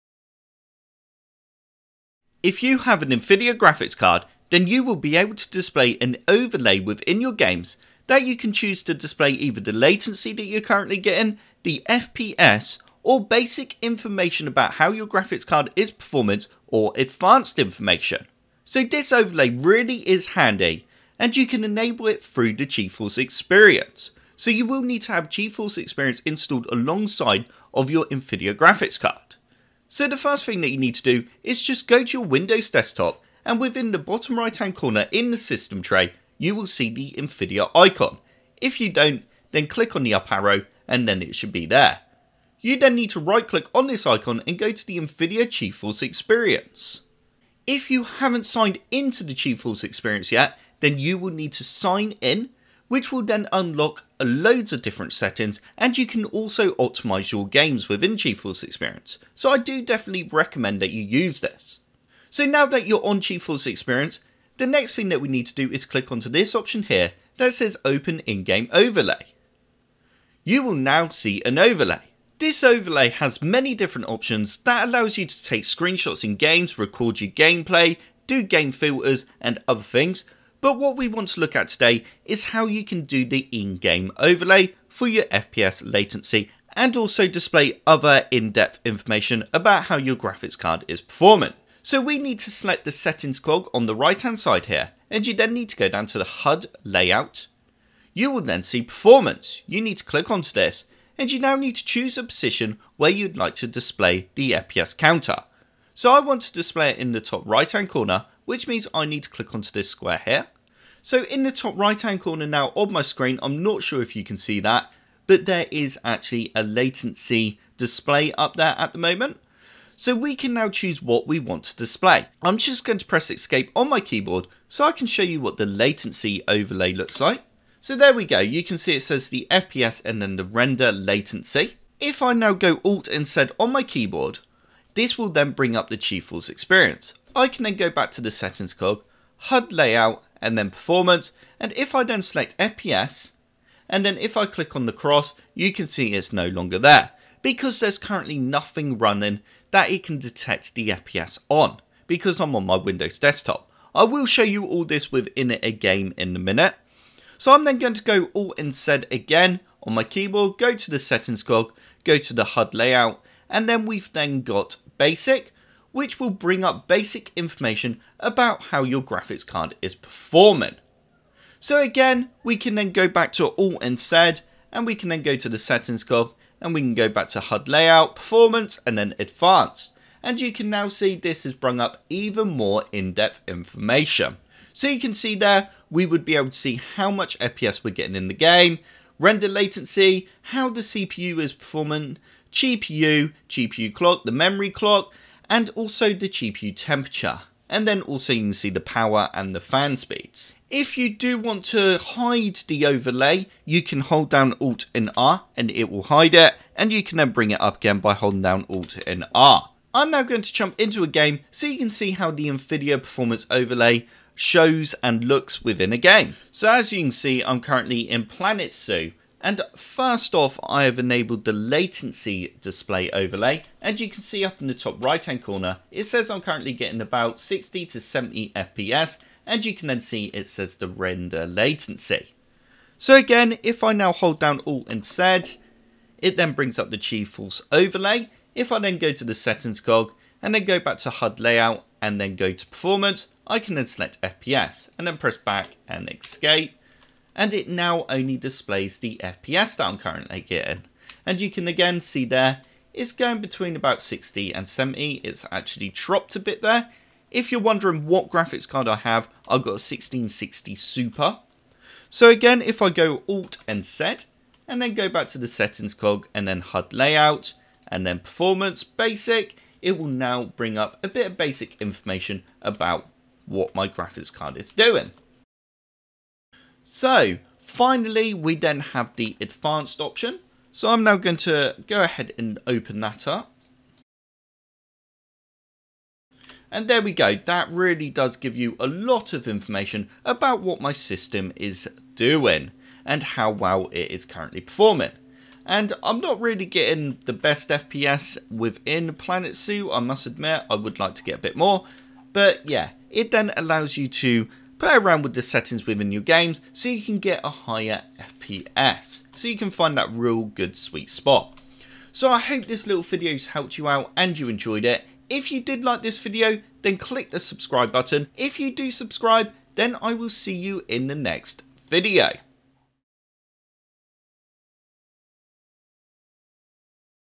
Enable NVIDIA GeForce FPS Overlay | In-Game Performance Overlay
How To Enable NVIDIA Performance Overlay

In this video, I will show you how to display the NVIDIA GeForce FPS counter. NVIDIA GeForce Experience has an option that allows us to enable an FPS counter, latency, and basic/advanced performance information about how our system is performing while playing games.

The NVIDIA Performance overlay will show GPU, FPS, 99% FPS, Render Latency, CPU Utilisation, GPU Utilisation, GPU Clock, Memory Clock, GPU Temperature, Fan Speed, GPU Power, and GPU Voltage. As you can see enabling the NVIDIA performance counter overlay in-game is a very handy feature, and one I personally like to have shown while I play my PC games.